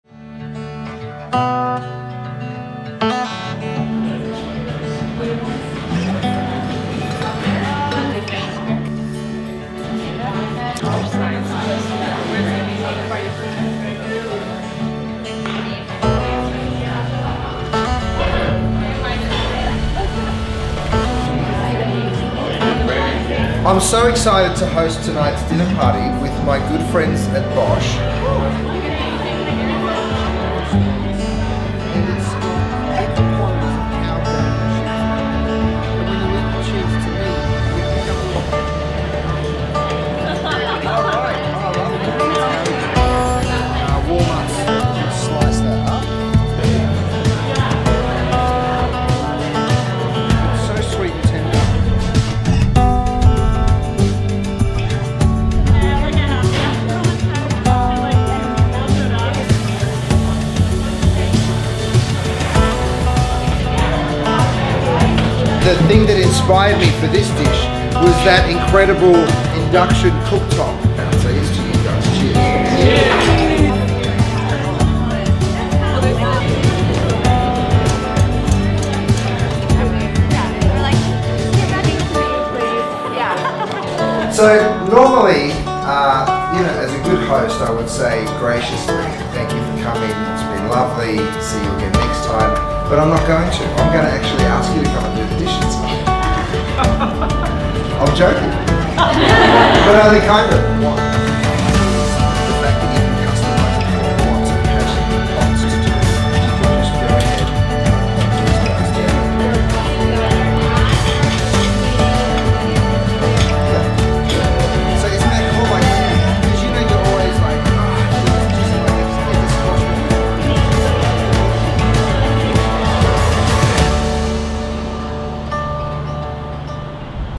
Bosch and Chef Curtis Stone Host A Sparkling Evening in the Hollywood Hills
Last month, Bosch and Chef Curtis Stone hosted 20 passionate foodies for a sparkling evening of food, laughter, and plenty of big surprises. Check out this video for an inside look at the delicious, seasonal meal that Chef prepared, and an exciting twist he unveiled at the end of the evening.